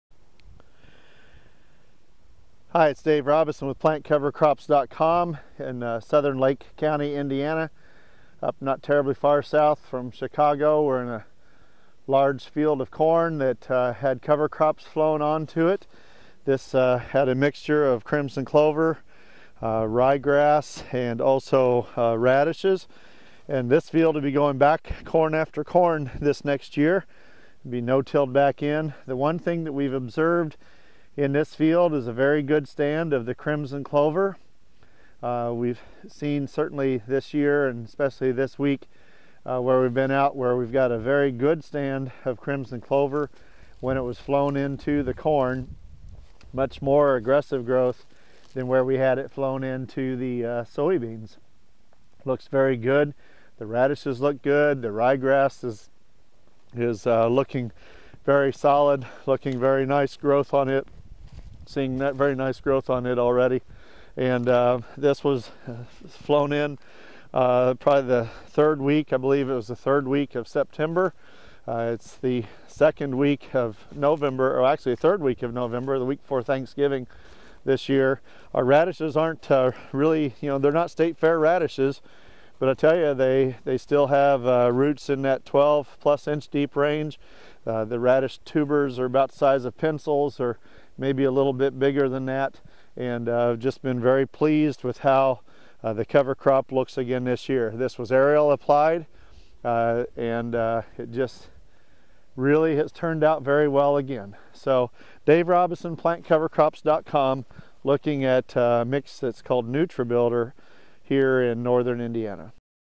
Cover crop report- Crimson Clover/Radish/Annual Ryegrass flown into corn-November 2011
This late November view of cover crops in NW Indiana shows a beautiful stand of crimson clover, radishes, and annual ryegrass.  This mix (called N-Vest NutriBuilder) had very consistent growth across the whole field. Particularly look at the very nice stand of crimson clover compared to where we have seen it in soybeans (where the clover was applied to 'too green" soybeans).